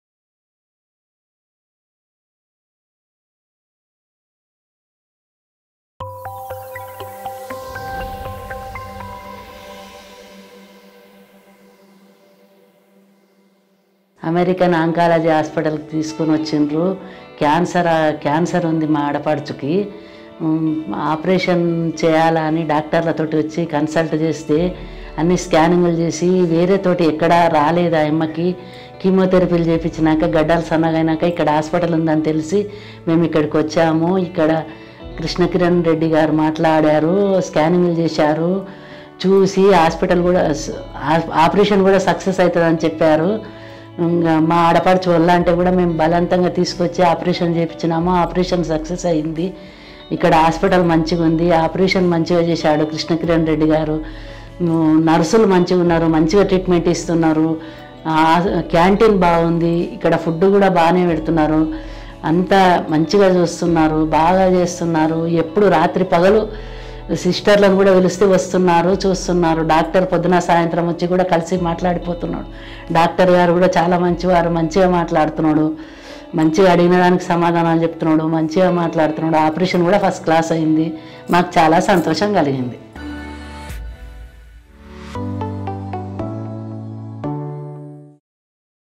Successful Treatment for Cervical Cancer During COVID19 at American Oncology Institute, Hyderabad
Anasuymma from Hyderabad underwent a successful treatment for Cervical Cancer at the American Oncology Institute, Hyderabad. Speaking about the treatment, her sister-in-law Janakamma says that they have had an excellent interaction with doctors and the nursing team exceptional care.

Answering all their queries promptly and meeting all their needs in time, the hospital provided excellent patient experience and the patient outcomes have been satisfactory. Despite the challenges of the corona virus pandemic, the hospital ensured that all the measures were taken to provide a safe environment for everyone.

Social distancing, adequate screening, and added impetus on sanitation were undertaken to make sure patients, staff, and visitors remained safe, and did not cause transmission of the corona virus.